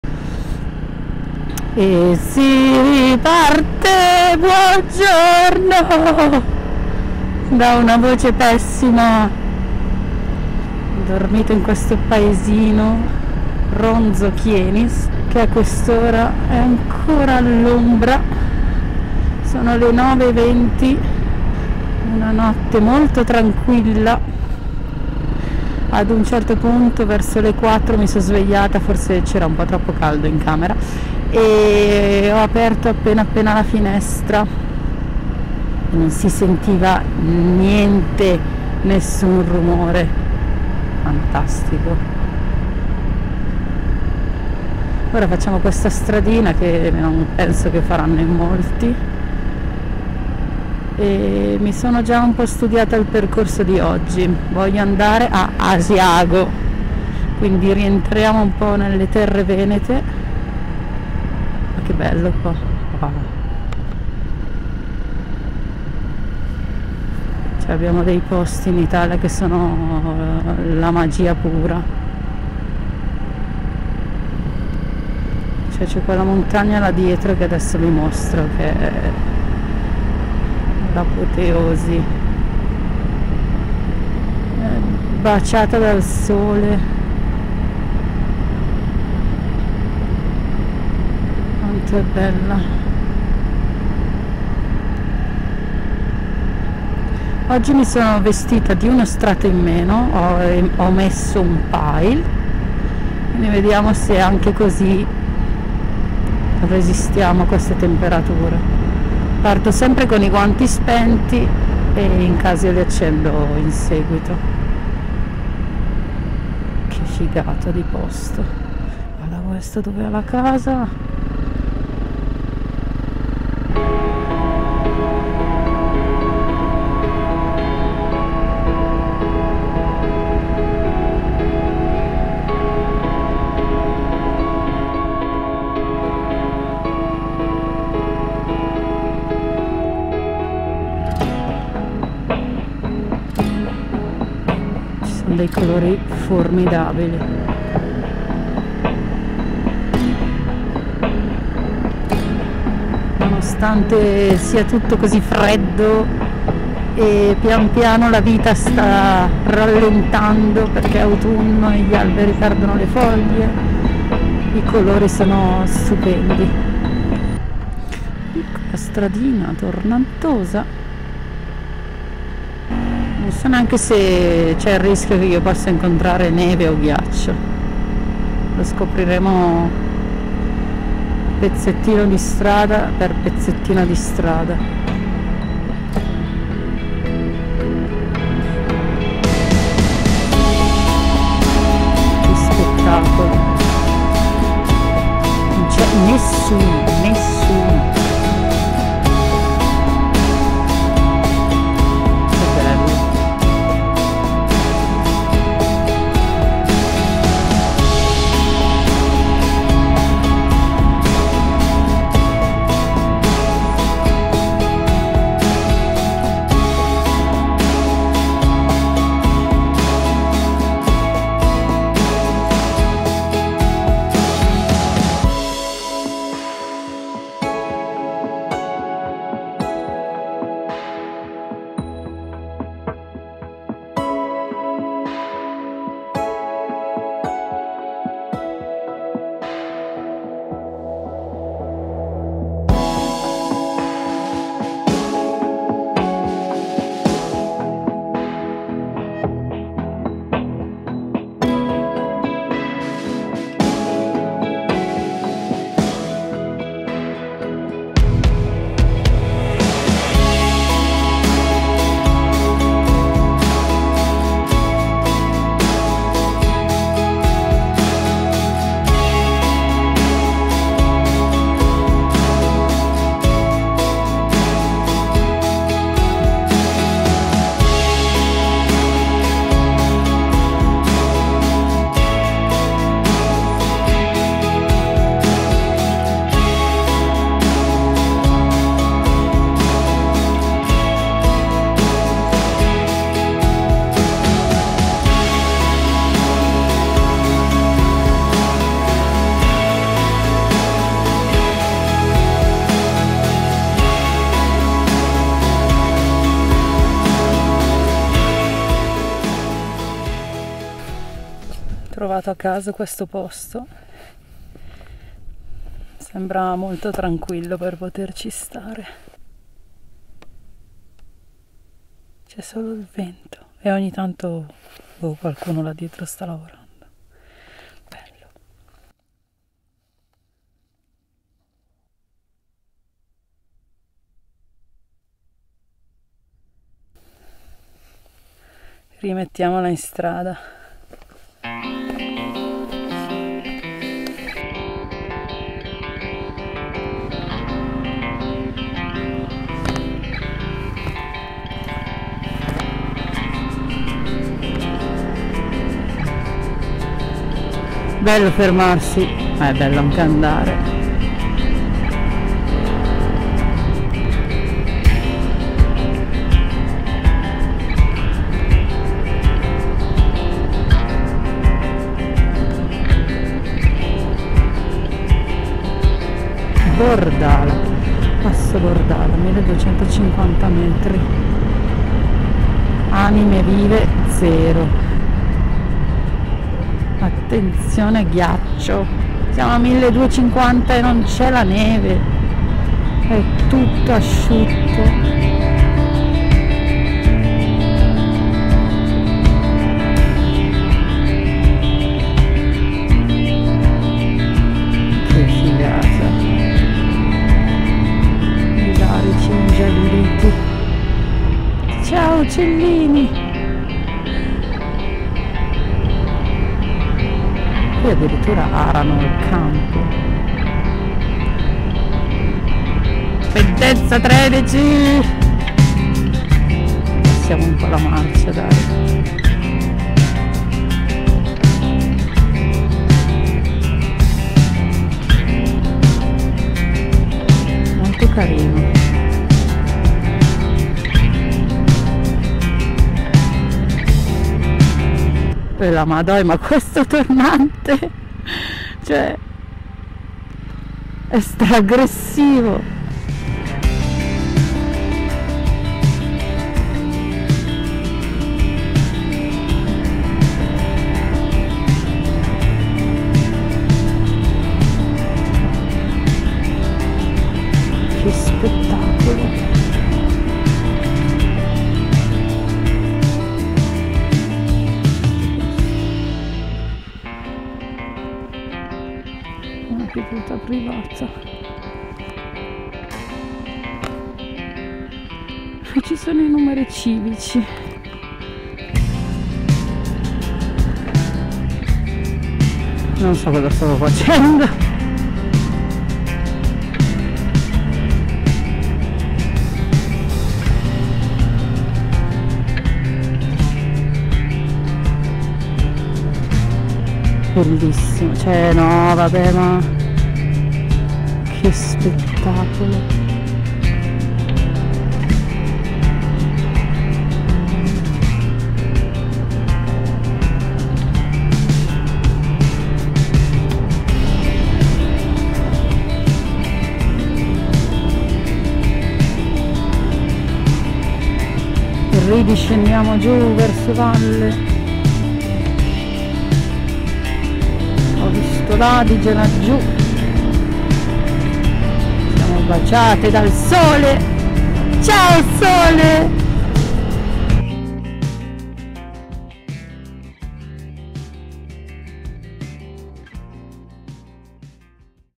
Risveglio al tranquillo e stretto passo Bordala - In moto col "freddo" - Parte 4
Secondo giorno di un piccolo giro autunnale tra i passi meno battuti e poco impegnativi del Trentino. Ogni volta questa provincia mi sa dare un senso di beatitudine e calma che difficilmente trovo altrove. Basta mettersi in strada, prenotare un albergo a caso e ti ritrovi su un passo qualsiasi, in questo caso il Bordala, a goderti la natura.
Il freddo mattutino si fa sentire e diventa essenziale accendere un mio recente acqusito: i guanti invernali della Clover Polar WP.

La mia moto:
Honda CRF 300 Rally